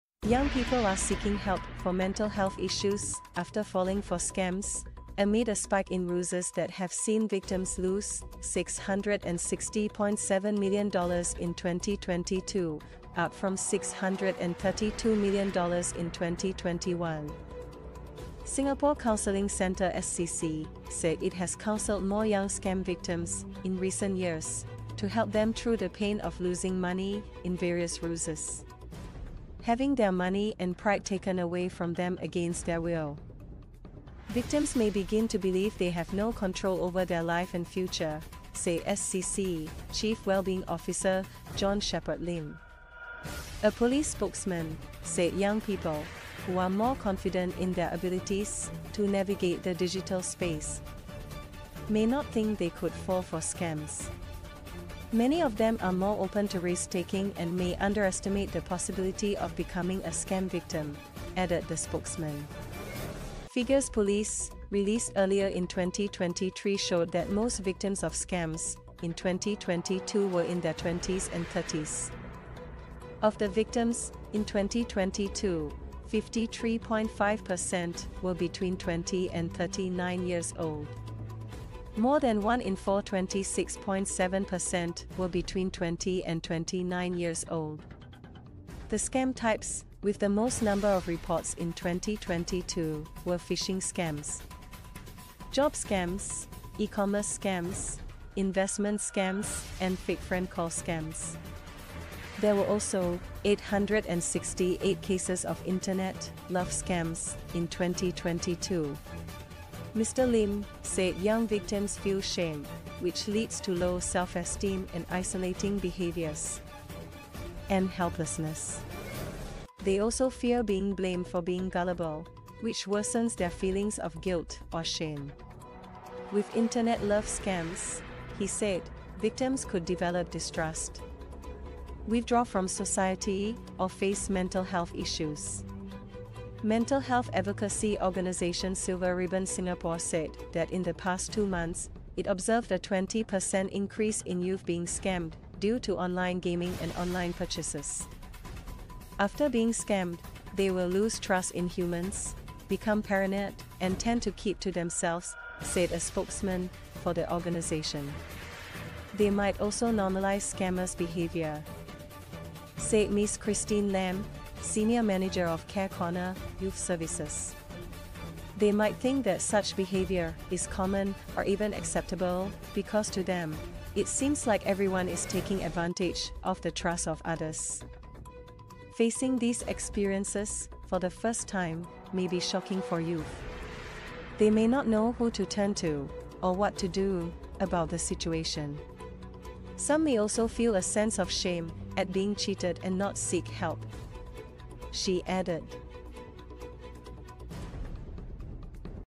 Young People Among Scam Victims Seeking Counselling After Falling for Ruses
- Young people in Singapore are increasingly falling victim to scams and seeking counseling for support.
- The number of young people seeking counseling after falling for scams has increased by 40% over the past three years.
- Scammers are targeting young people through various methods, including online platforms and social media.
- The scams often involve promises of quick money or attractive investment opportunities.
- Many young people are unaware of the signs of scams and are easily lured by the promises made by scammers.
- Counseling services are being provided to help victims cope with the emotional and psychological impact of being scammed.